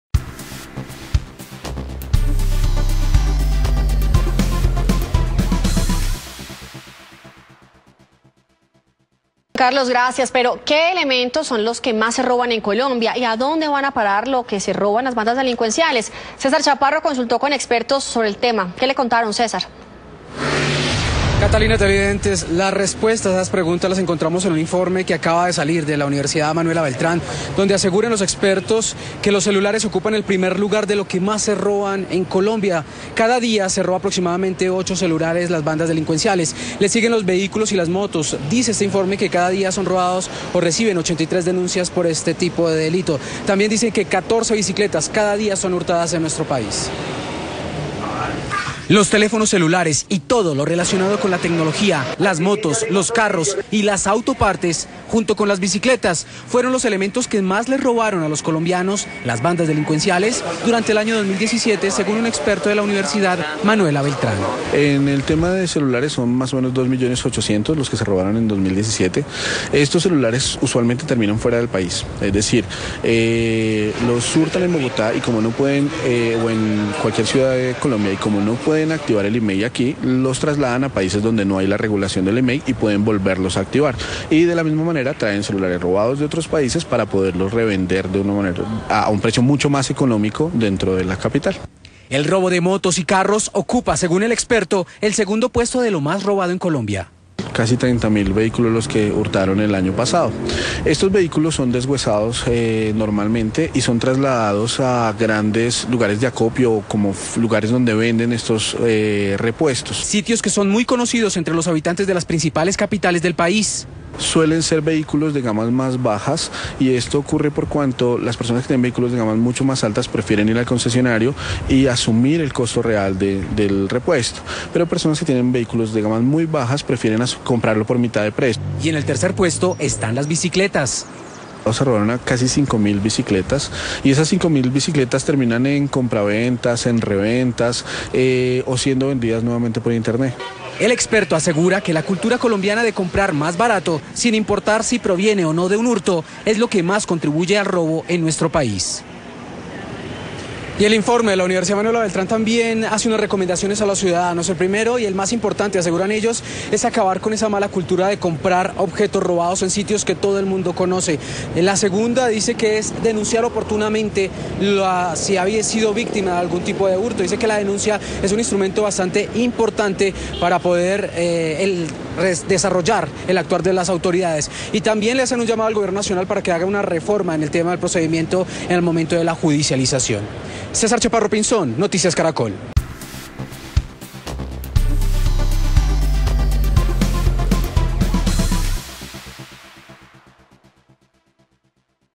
UMBenlosMedios
¿Sabes a dónde va parar lo robado? El Dr. Jhon Carvajal director del Programa de Derecho de la UMB, nos da un importante panorama de los objetos más robados en el país, en donde el primer lugar lo ocupa el rapto de celulares.